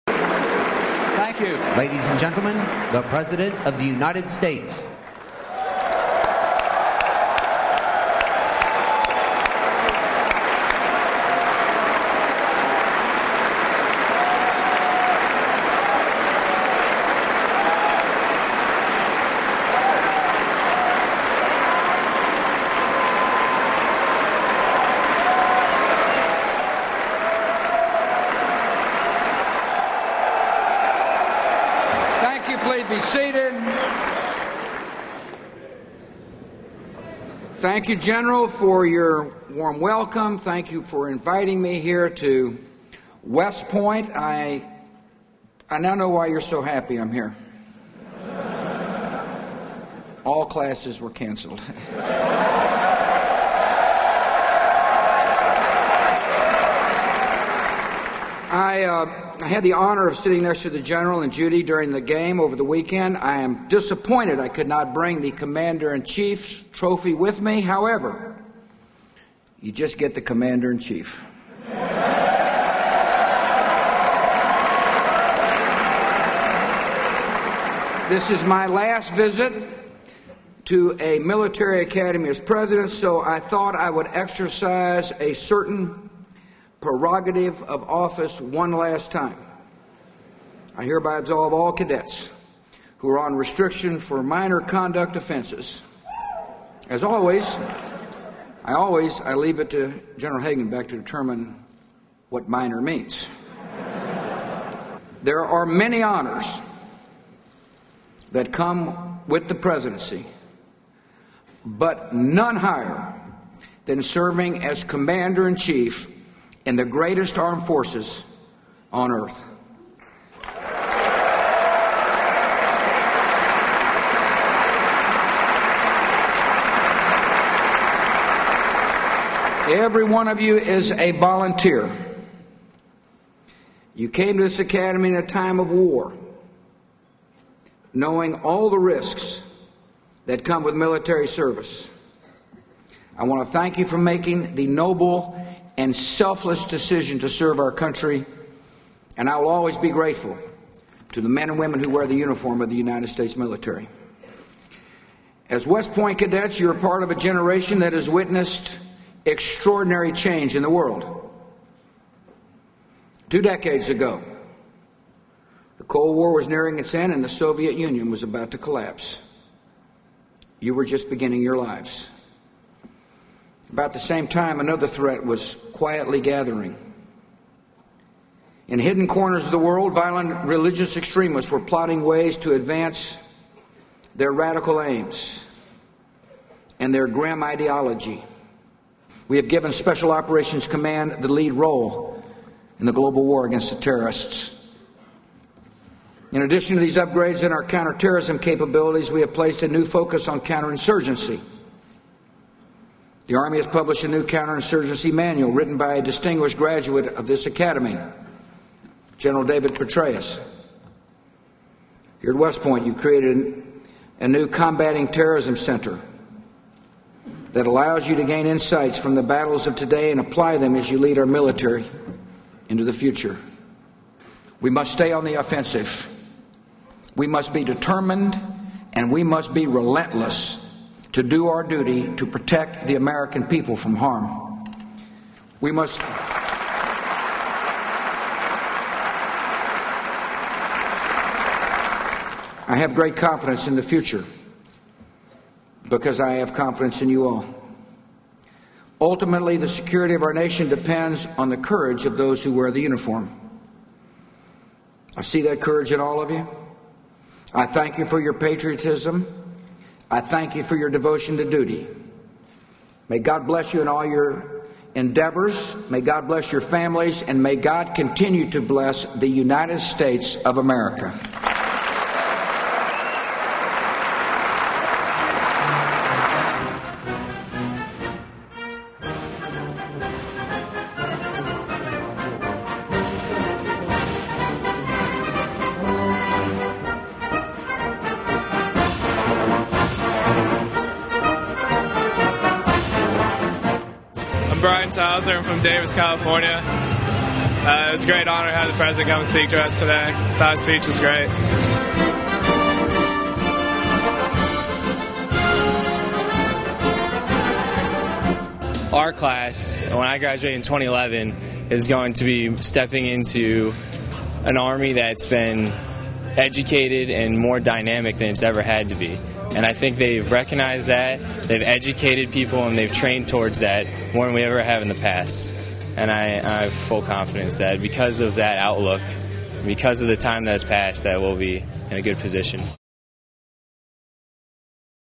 President Bush speaks at West Point
for more West Point videos/photos/articles. Addressing the corps of cadets at the U.S. Military Academy, the president said that after the Sept. 11, 2001 terrorist attacks, the U.S. saw the urgency of staying a step ahead of our enemies, a policy reflected in what is known as the Bush Doctrine. Bush said terrorism continues to pose a serious challenge, as seen in the attacks last month in Mumbai, but that the terrorists have been weakened. (AP news)